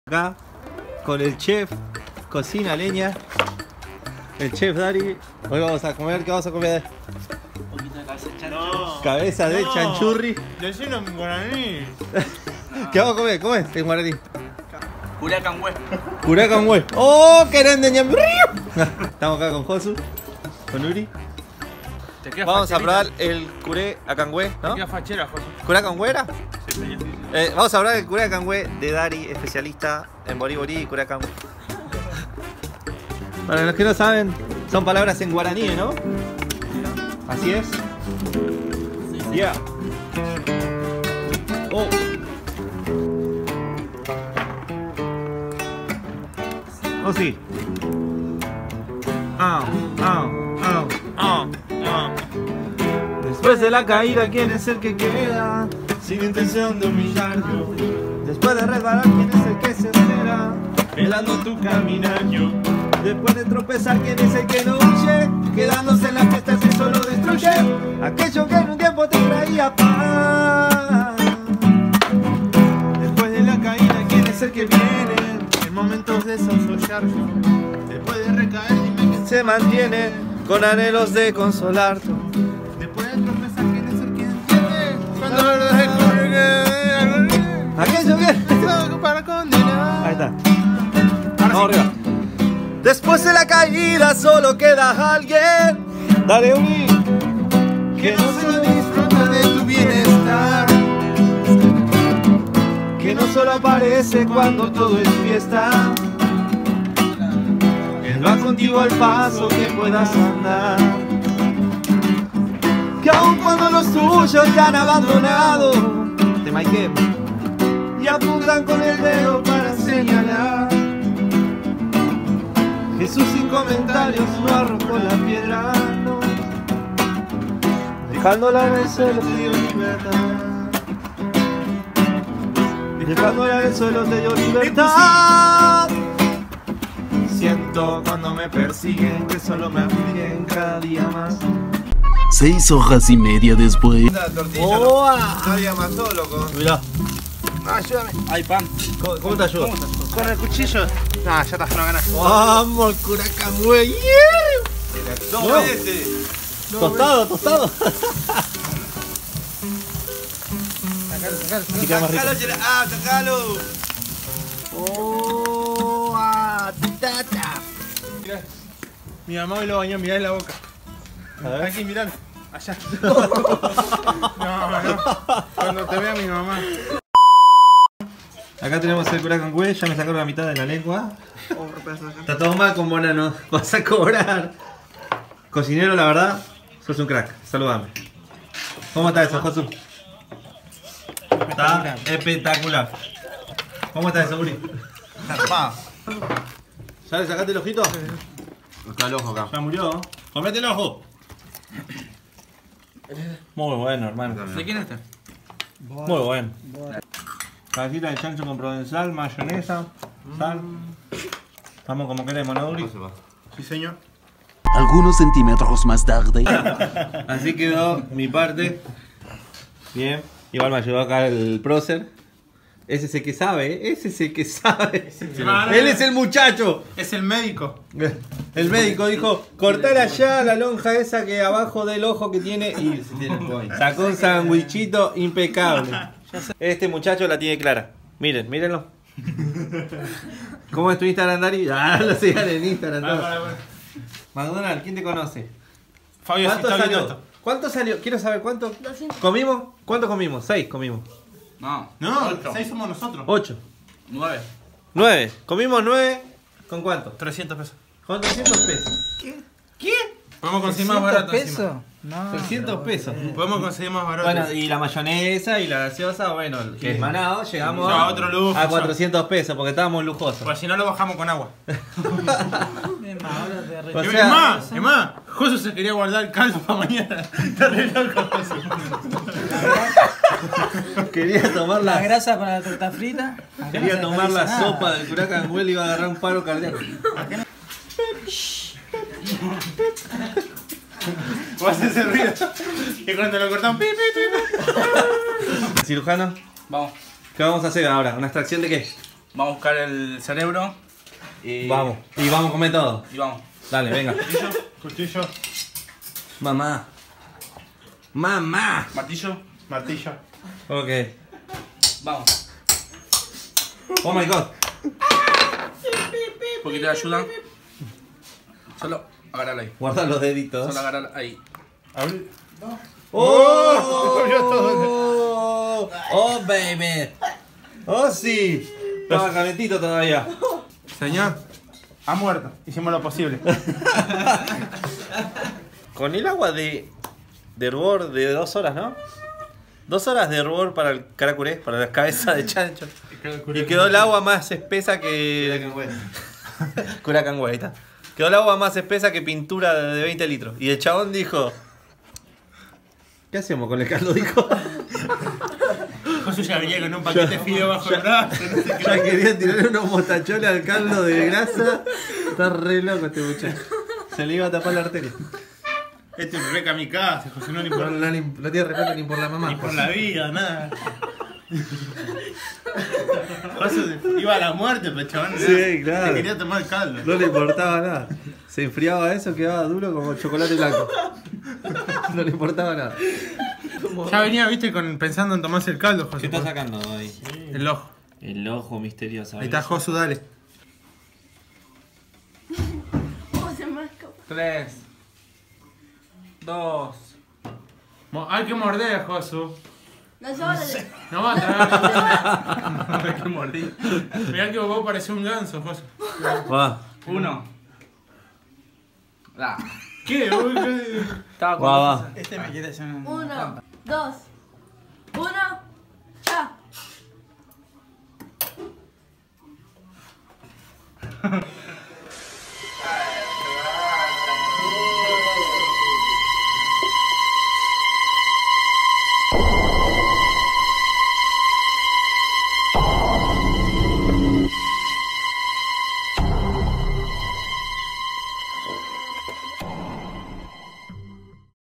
Comiendo CABEZA DE CHANCHO con amigos KURE ACANGÜE 🐽
Después de mucho tiempo nos juntamos a comer algo jejej y bueno...
Lo único que alcanzo fue esto 🤣😅💪🏻